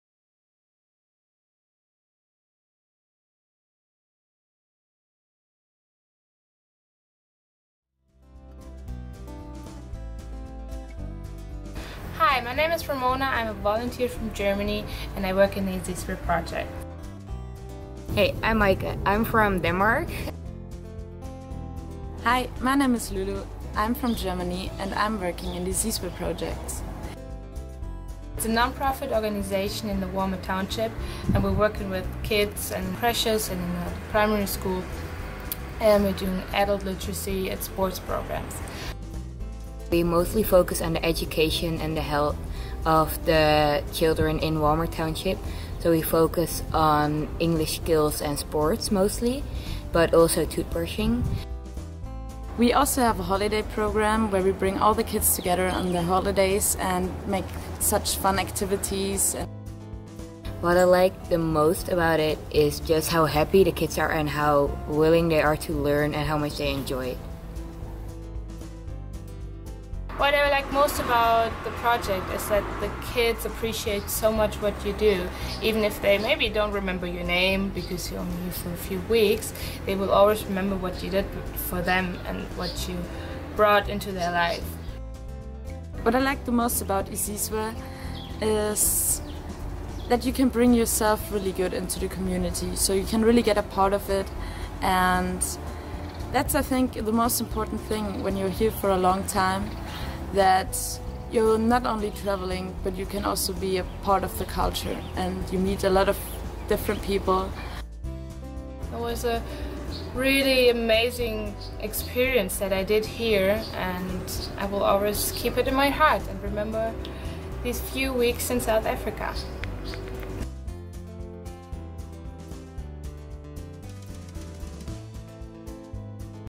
Volunteer Testimonials Izizwe Projects South Africa
Need some motivation for your volunteer trip to South Africa? Listen to these awesome Khaya Volunteer testimonials and get a glimpse into the well known Izizwe Projects.

Volunteer with Children at Izizwe Projects South Africa

Volunteers will work in vibrant Walmer township with the locals, while helping with the care and teaching of young children.
The project consists of several programs all run and supervised by the Izizwe Volunteer Coordinators, who are all volunteers themselves and live in Walmer Township. The diversity of possibilities creates an opportunity for volunteers to suggest preferences, for age groups and specific personal interest to be involved in.